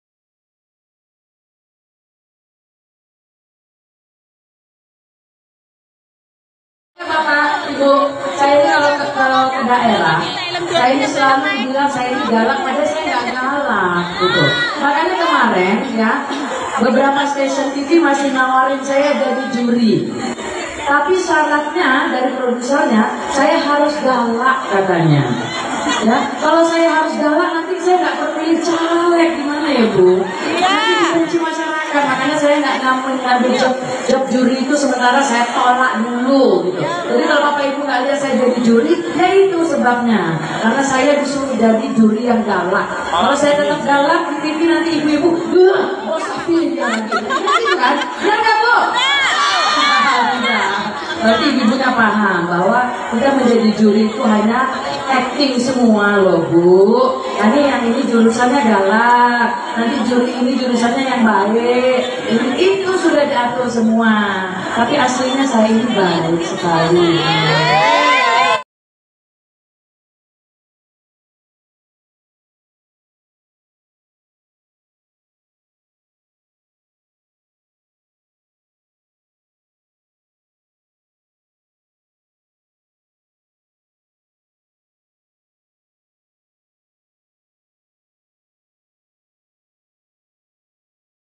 Iyeth Bustami Ungkap Alasan Tak Lagi Menjadi Juri, Saat Tampil Di Lubuklinggau
Iyeth Bustami Ungkap Alasan Tak Lagi Menjuri, Saat Tampil Di Lubuklinggau